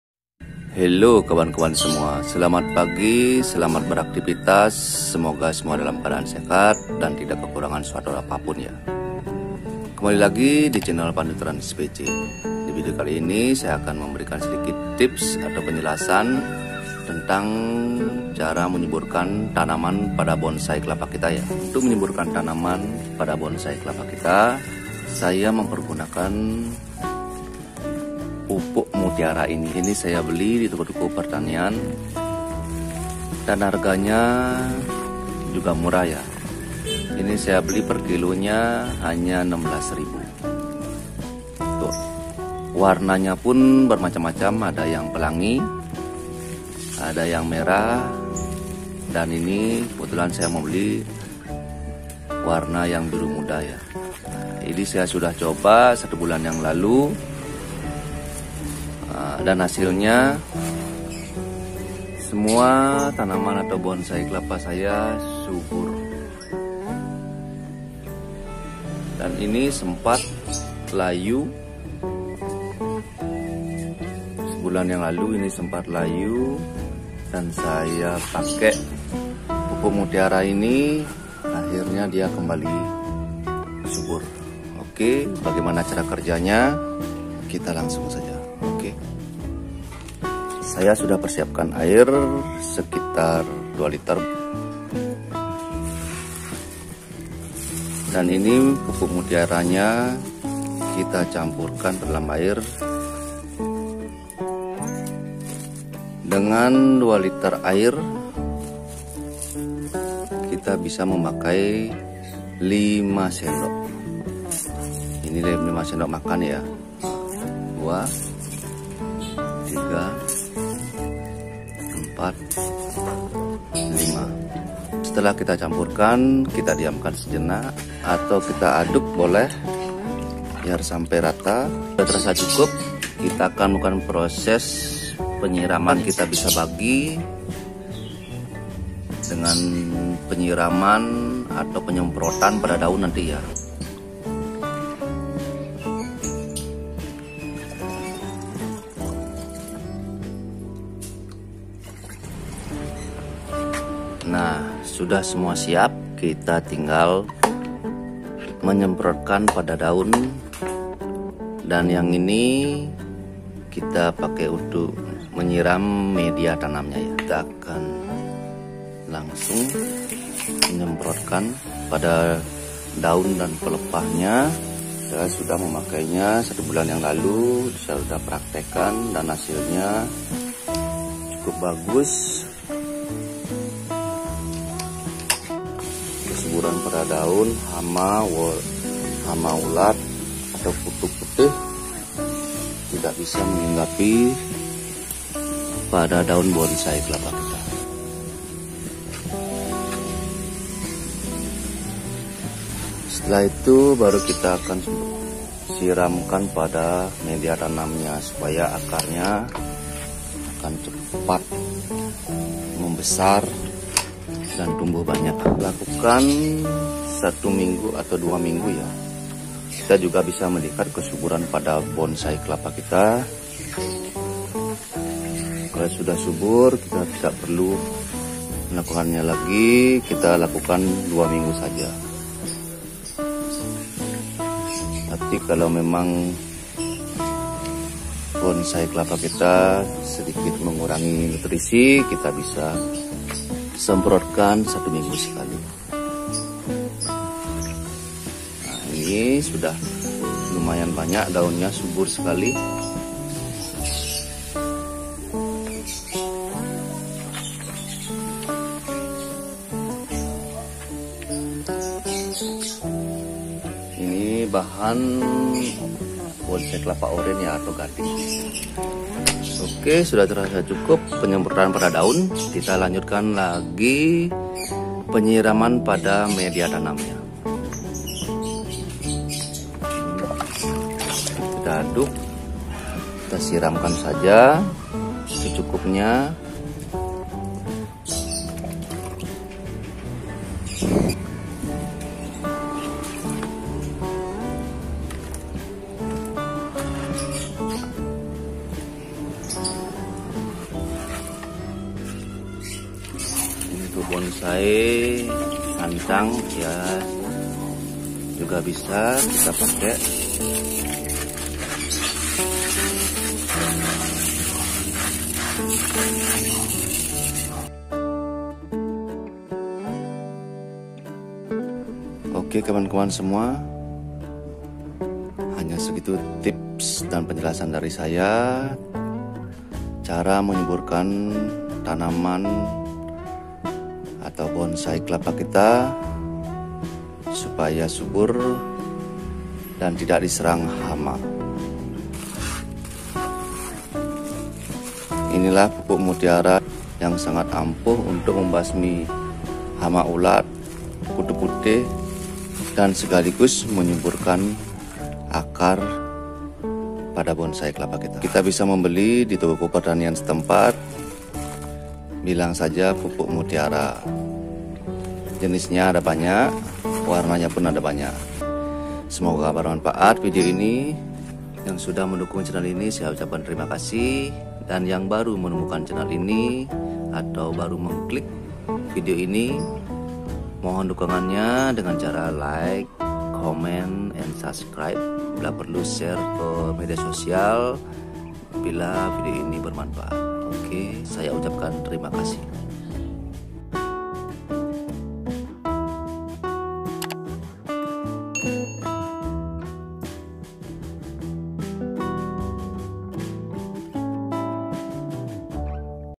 PENYUBUR TANAMAN BONSAI KELAPA DENGAN PUPUK MUTIARA..PANDU TRANS PC.
penyubur tanaman bonsai kelapa dengan pupuk mutiara. untuk menyuburkan tanaman tentunya kita memerlukan pupuk, tujuannya agar tanaman kita menjadi subur. sesuai pengalaman saya, salah satu pupuk yang bagus untuk menyuburkan bonsai kelapa adalah pupuk mutiara. sangat gampang kita cari di toko- toko pertanian. dan caranya juga sangat mudah. untuk jelasnya kita simak video ini sampai habis.